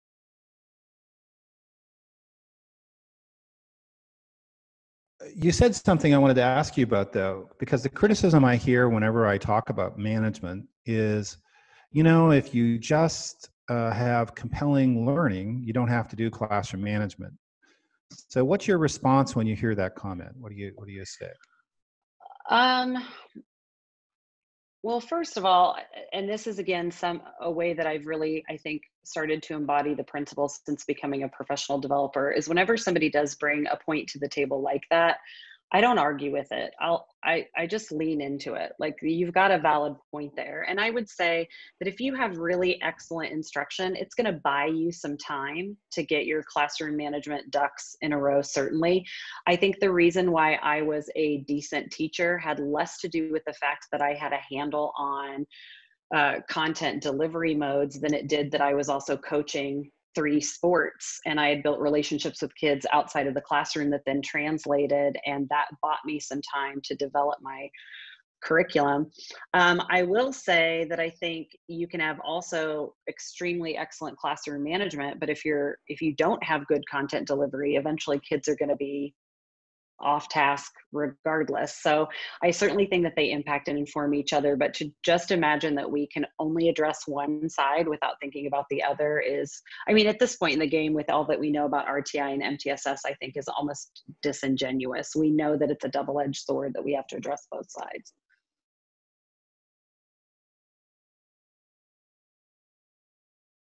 TLC 2021 Presenter Tricia Skyles on Compelling Learning and Classroom Management
Tricia Skyles and Jim Knight discuss the topic of compelling learning and classroom management.

Enjoy more conversations like this at the Teaching Learning Coaching conference, Oct 2-4, 2022